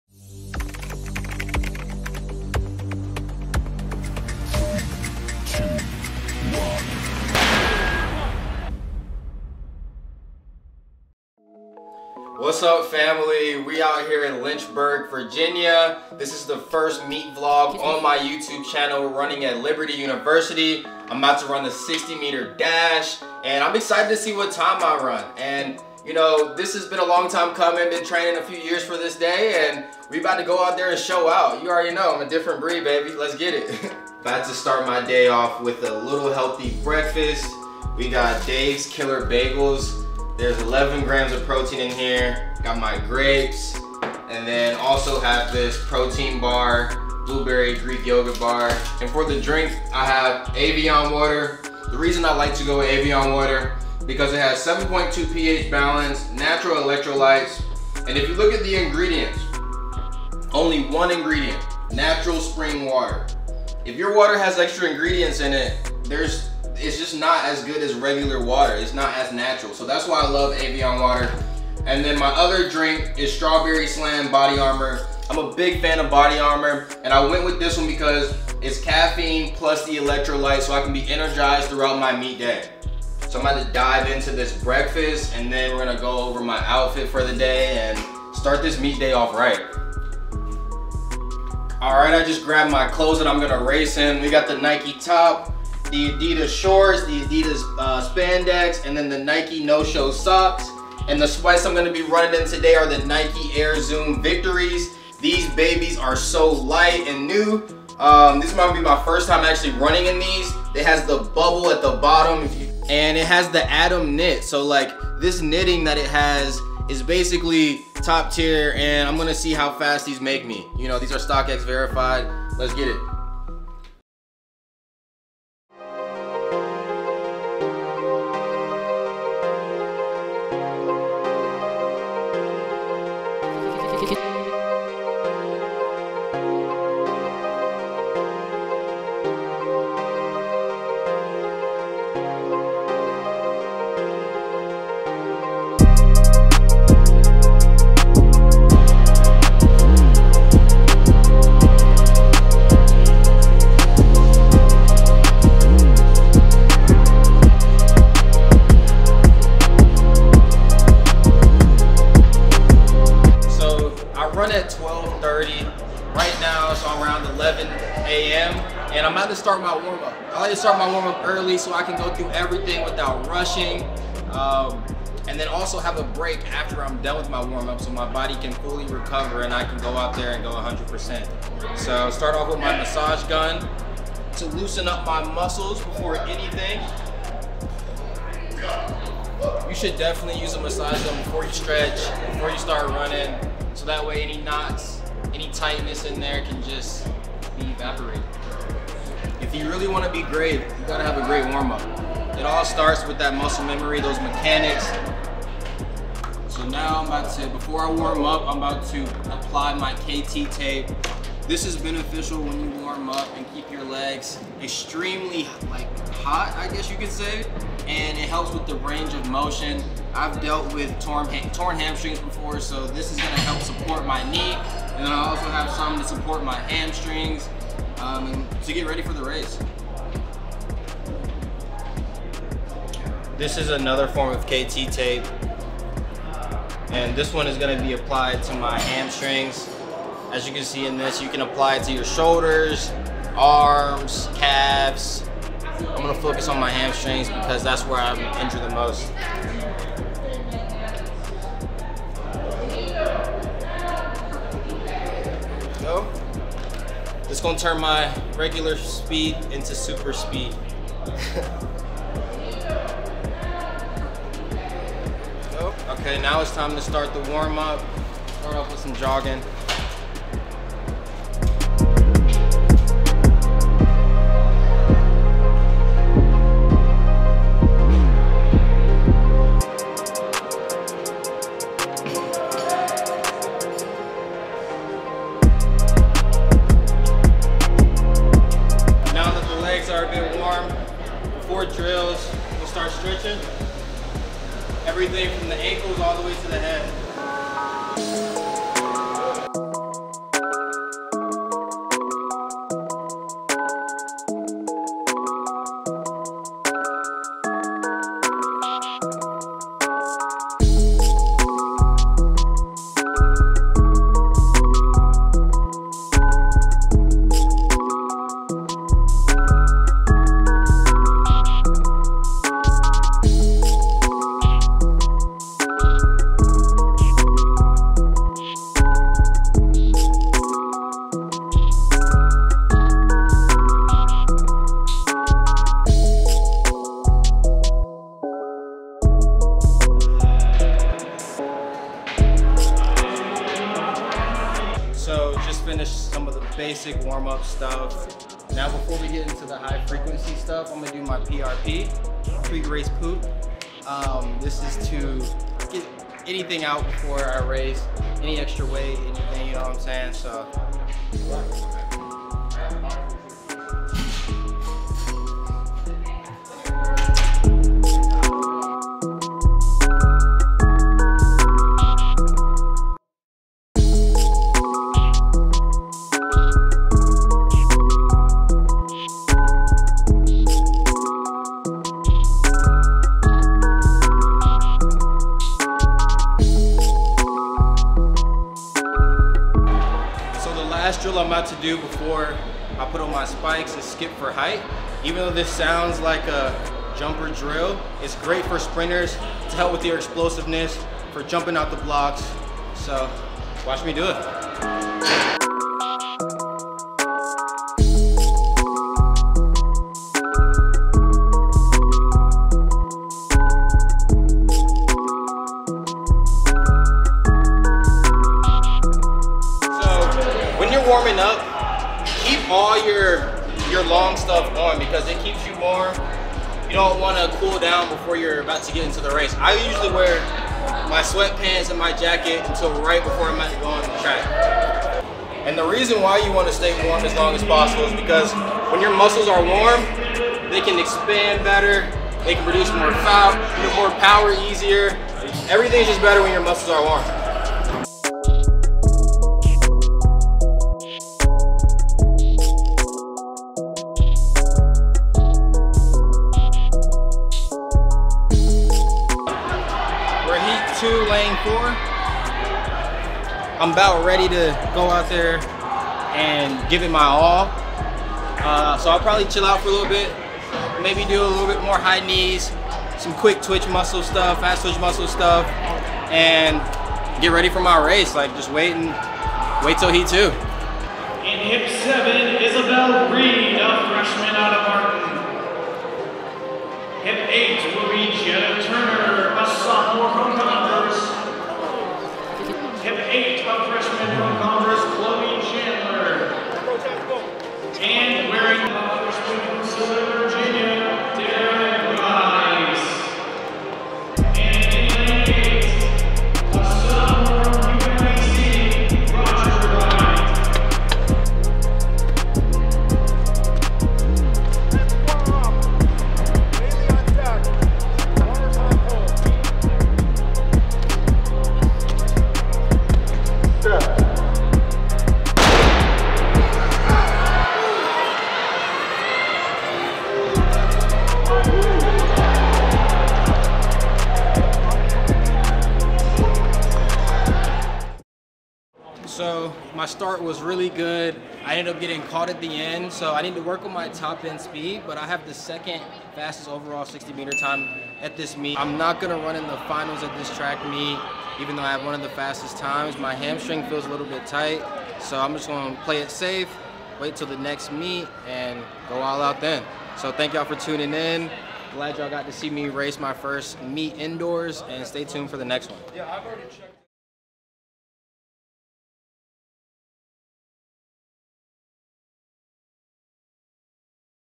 Track Meet Vlog. Epic Viral 60 Meter Race!!
Go through a meet day with me and see how I qualify for finals with a shocking ending! This was the first time ever doing something like this, let me know what you guys think & let me know if you want me to vlog an outdoor meet this season!